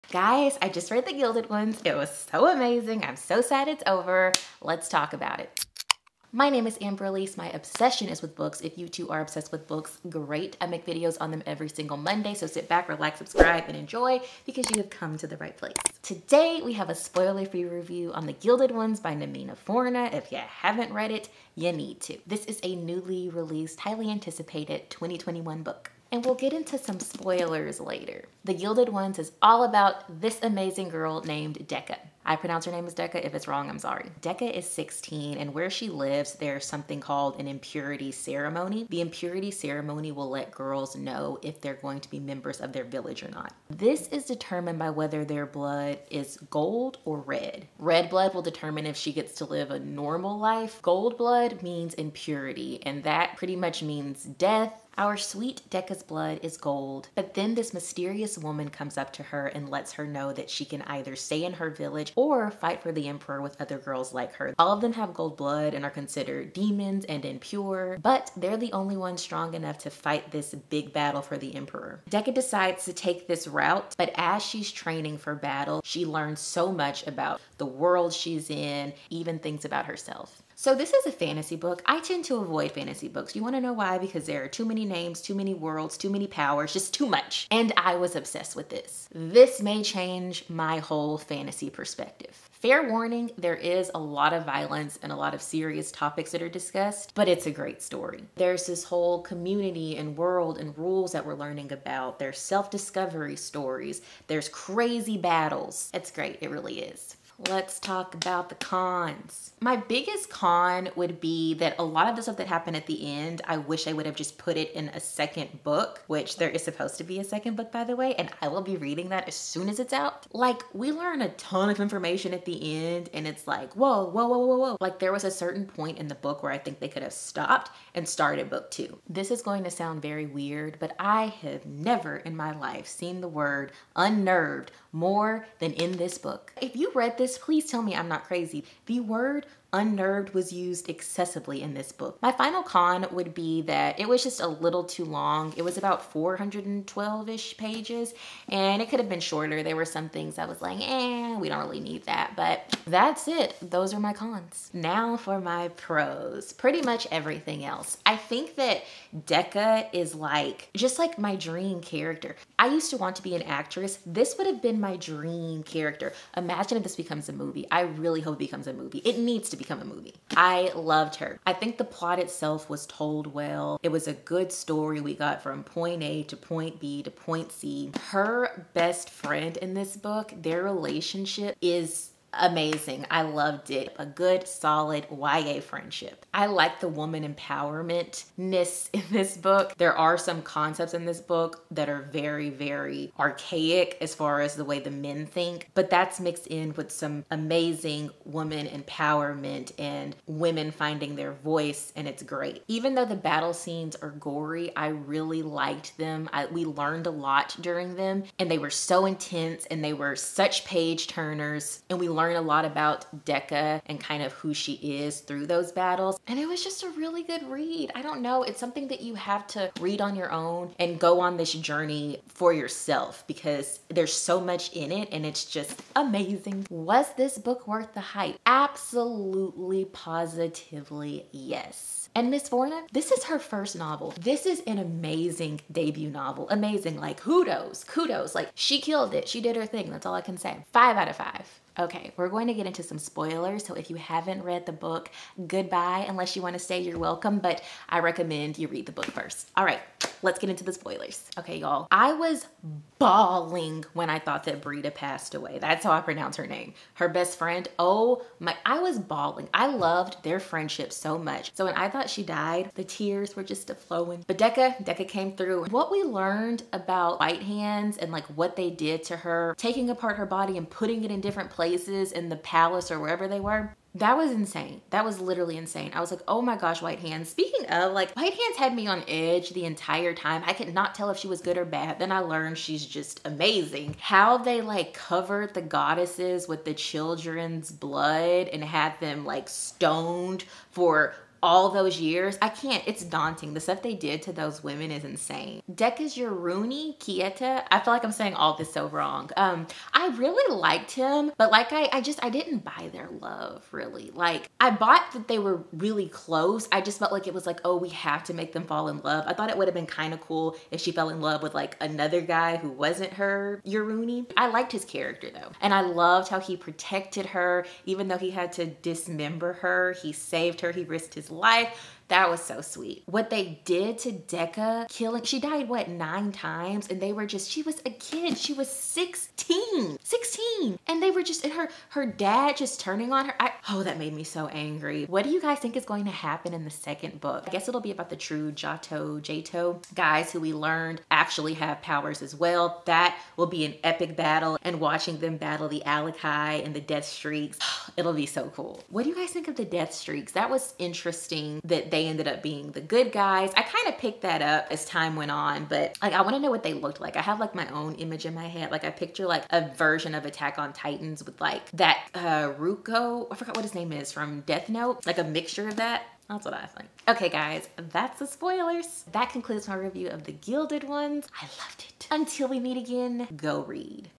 The Gilded Ones | Book Review | New Book Release 2021 | Spoiler Free
Hi there!

I just read The Gilded Ones by Namina Forna and I thought it was AMAZING! The Gilded Ones is a fantasy, young adult book. The Gilded Ones is a new 2021 book that was recently released. It was highly anticipated, and I am happy to report that it lived up to the hype! If you would like to know my spoiler free thoughts on The Gilded Ones by Namina Forna, this video contains my book review on this highly anticipated book. Enjoy!

Timecodes
0:00 - Introduction
0:28 - Summary of the Book
1:20 - My Overview/Thoughts of the Book
1:47 - Cons of this Book
2:34 - Pros of this Book
3:46 - Does it Live Up to the Hype?
4:00 - Rating I Would Give this Book
4:02 - Spoilers
6:07 - Final Remarks

Book(s) Mentioned in this Video:

The Gilded Ones